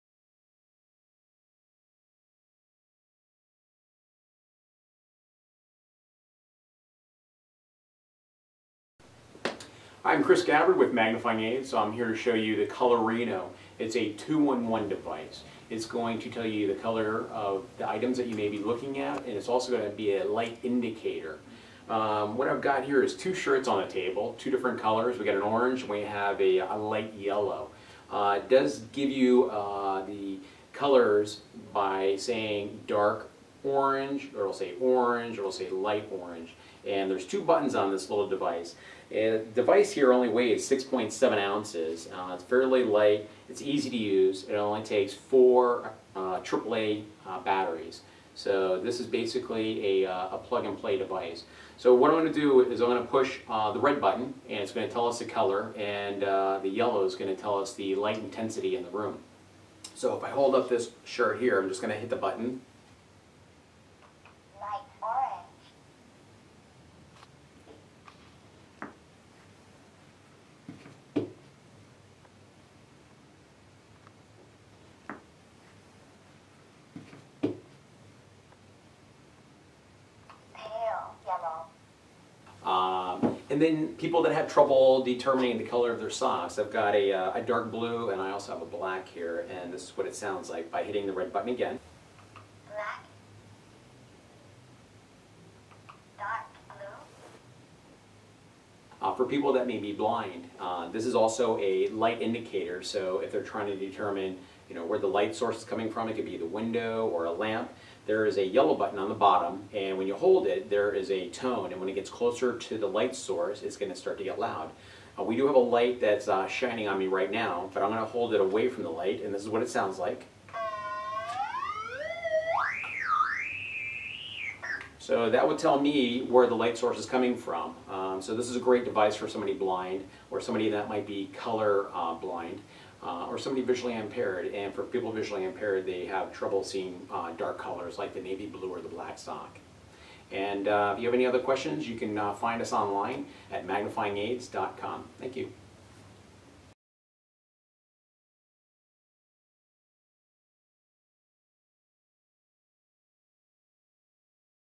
Colorino Color Identifier & Light Detector
Pocket sized 2-in-1 unit is the ideal Color Identifier/Light Detector for the blind and colorblind. Discerns and clearly announces the entire color spectrum of more than 150 shadings - not just red, for example, but pale red, light red, dark red, as well as intense red. Also detects the presence of light and indicates intensity and proximity by audio signal. Volume control; earphone jack and zippered carrying case. Unit measures: 4" x 2". Uses 2 AAA batteries, included. Weight: 6.7 oz. #756220